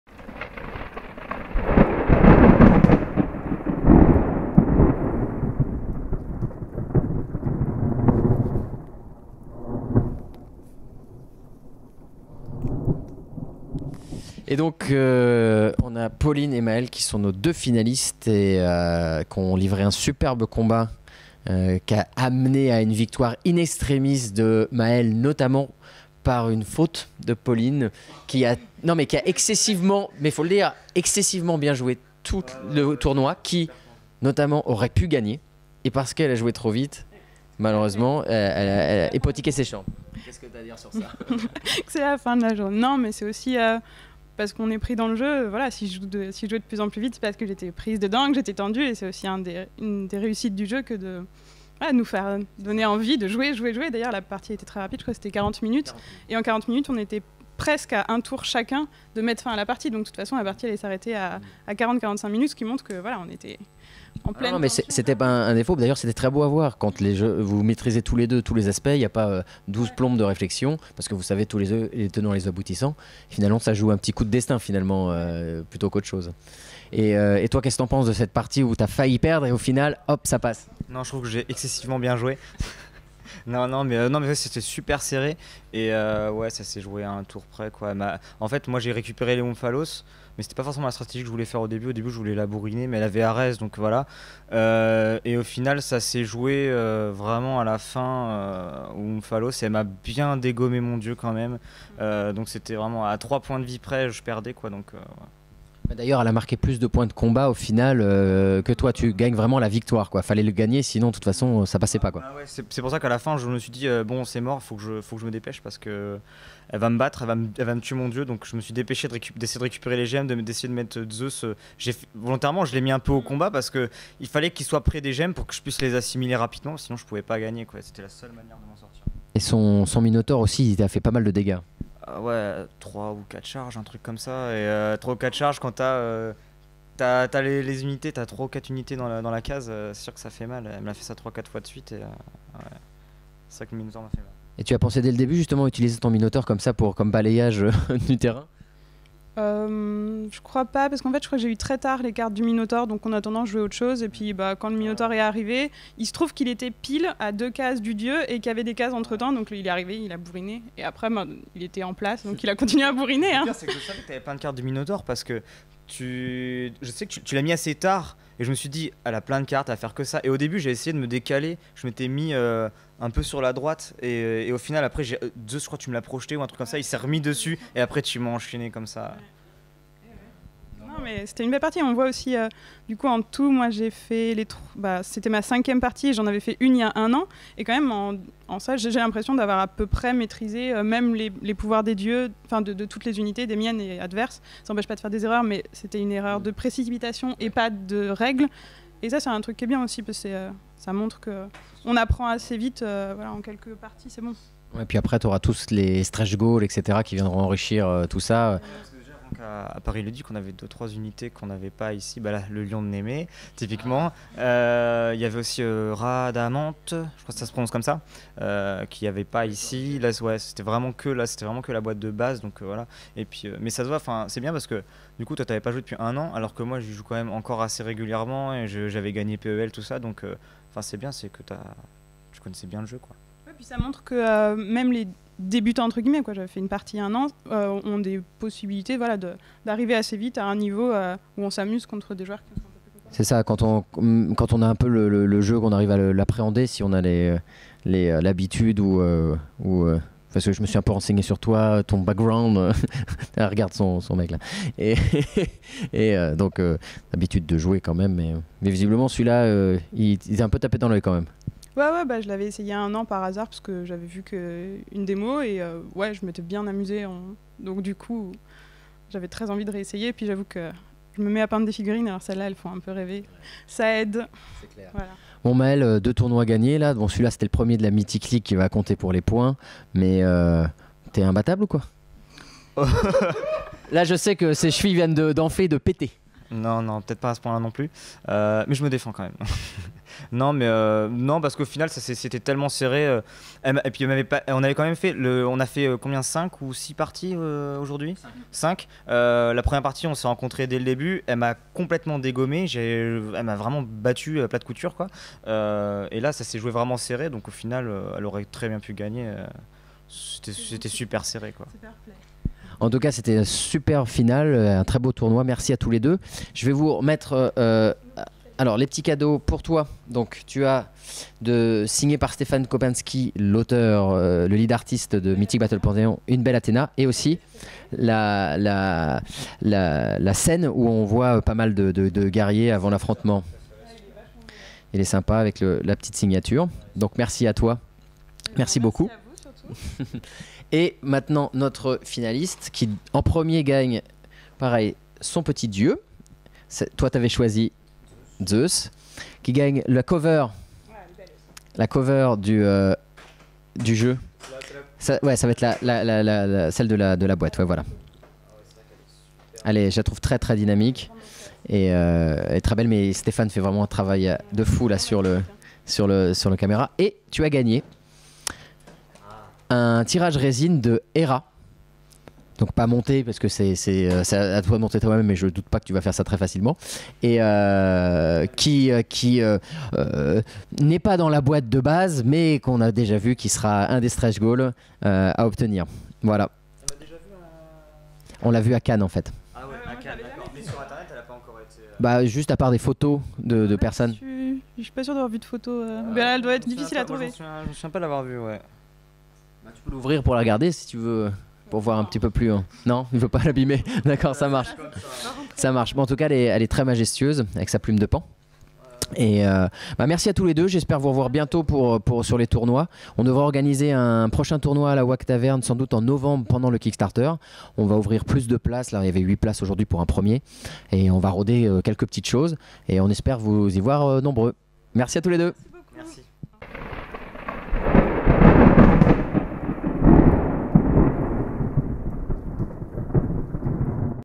Interview finalistes tournoi waaagh taverne (01/10/16) [Mythic Battles Pantheon]
Le 01 octobre 2016 s'est tenu le premier tournoi de la Mythic League !
Au menu, 8 joueurs pour un tournoi en draft Core Box, 3 rondes suisses en temps limité, demies et finales.
C'est Maël qui l'emporte in extremis devant Pauline au terme d'une superbe rencontre ! Ce fut une très belle après-midi et nous remettrons cela très bientôt ! Plus d'informations à venir sur notre page Facebook.

Mythic Battles Pantheon est un jeu de société avec figurines dont le financement sur Kickstarter démarre le 1er Novembre 2016.